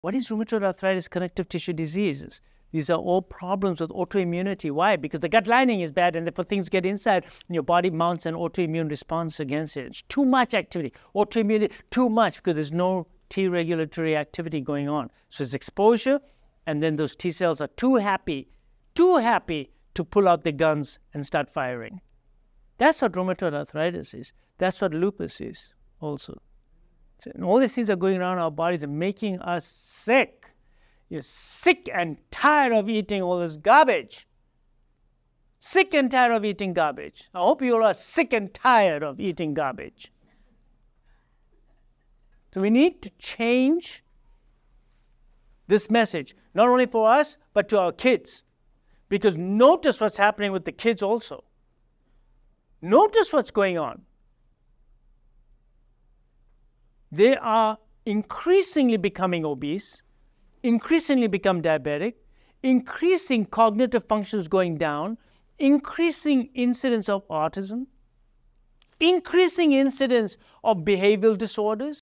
Autoimmune Diseases: Gut Health & How Food Messes Up Your Body!
Explore the critical link between autoimmune diseases, gut health, and overall well-being! We delve into the impact of a compromised gut lining, highlighting rheumatoid arthritis, lupus, and the consequences of poor dietary choices. Discover the vital importance of changing our message for a healthier future, especially for our kids, combating rising issues like obesity, diabetes, and cognitive decline. Learn how to improve health!